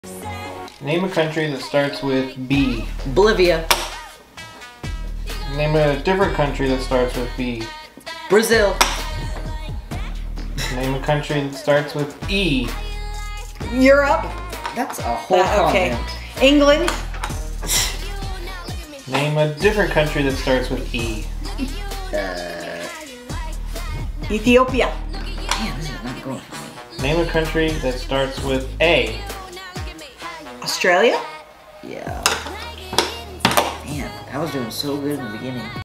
Country Trivia! 😲 Mom vs Son! TicTacToe!
Tic Tac Toe Country Trivia with Mom vs Son! Play along with us and see who wins!

Game/Challenge Props:
Incohearent Family
Uno Cards